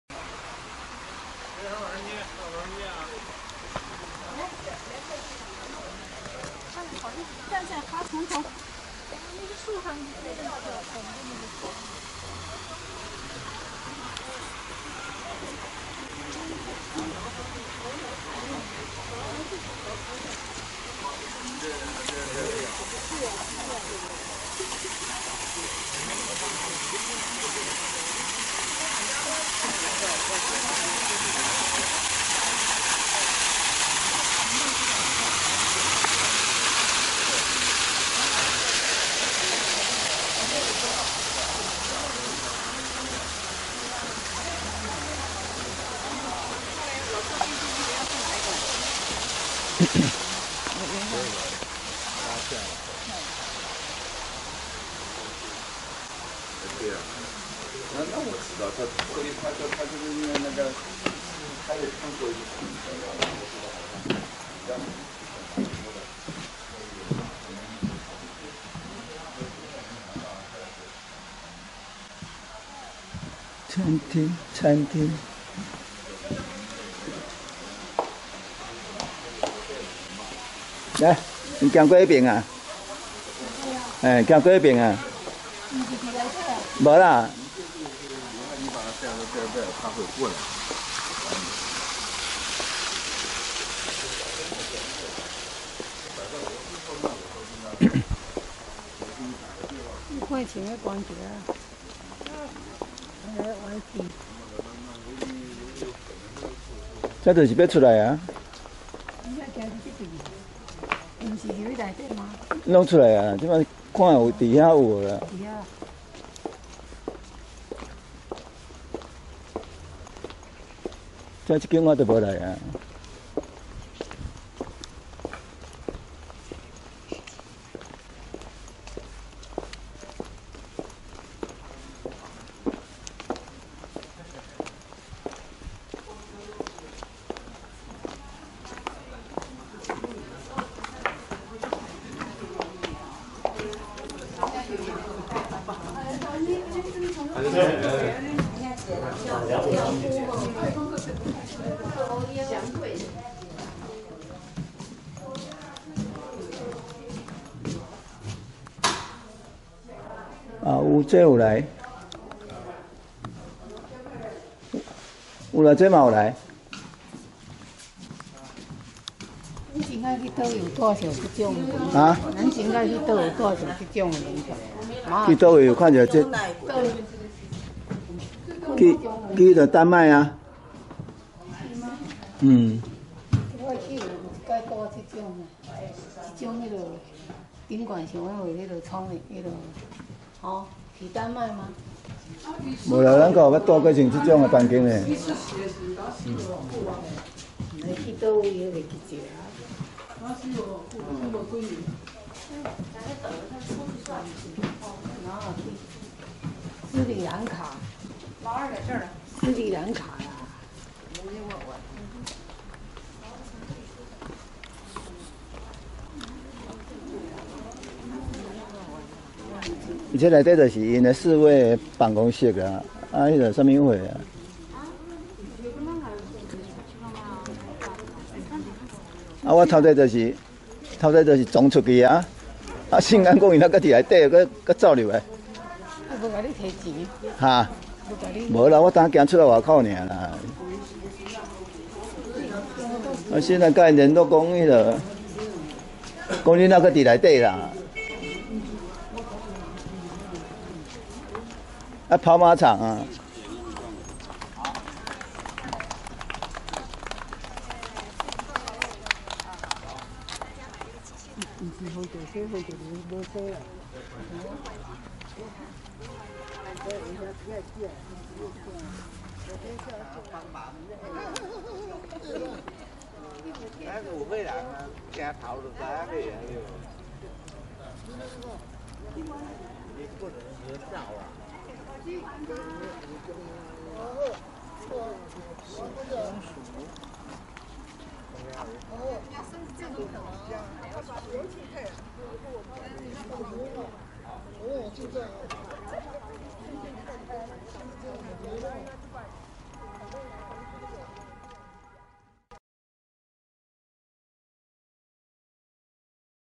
大陸東北吉林長春偽滿洲國皇宮19/21西御花園 Imperial Palace of Manchukuo Changchun, Jilin (China)
緝熙樓院落西側為御花園,有兩座竹亭,一座土山,花園北面為平房植秀軒,西套間曾是溥儀二妹韞龢及妹夫鄭廣元住所,後改為宮廷學生讀書處和桌球室.植秀軒之北為暢春軒,曾為溥儀四妹韞嫻及五妹韞馨住所.西花園南牆有連排平房,東半部為漿洗房,西半部為書庫,房前為網球場,後改為馬場.